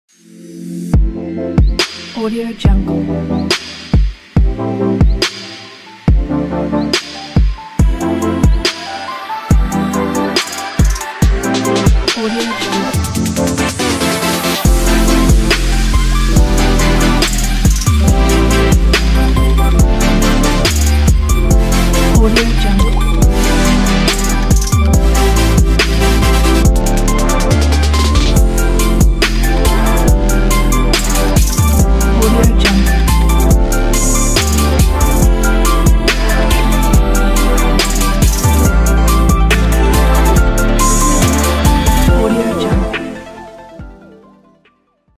Slideshow Fashion Slideshow for After Effects 2024
Hello! Here’s my new project Fashion Slideshow – After Effects!

PROJECT FEATURES:

Fast Render
Works with image or video
Works with After Effects 2022 and above